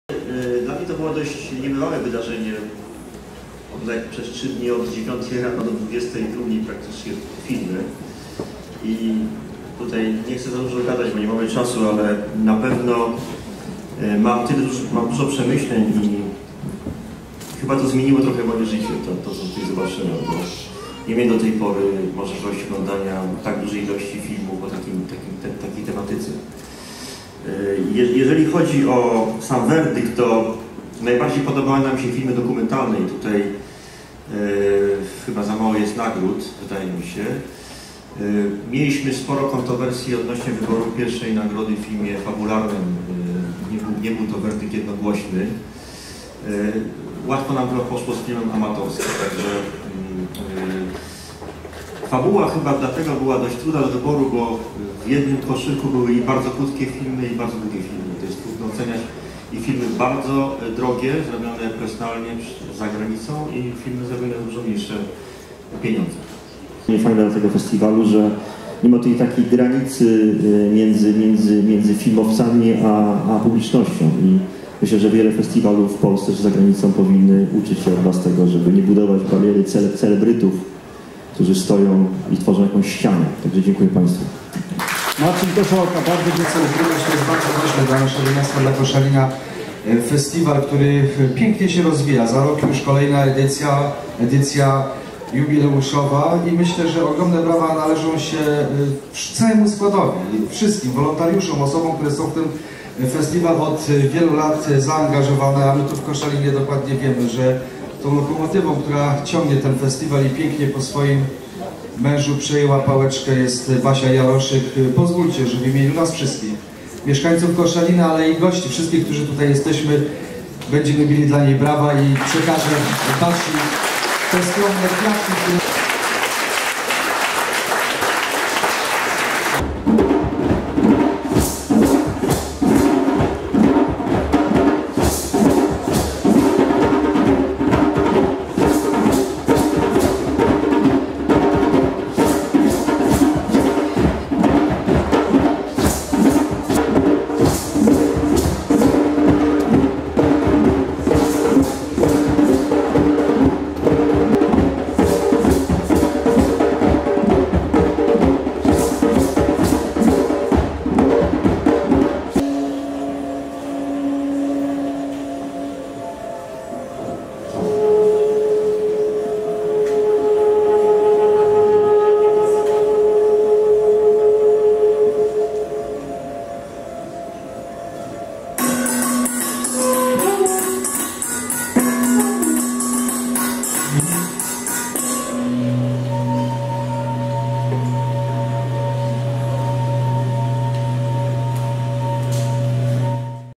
Zakończenie Festiwalu Integracja Ty i Ja 2017 w Koszalinie
W sobotę w Koszalinie zakończył się 14. Europejski Festiwal Filmowy Integracja Ty i Ja.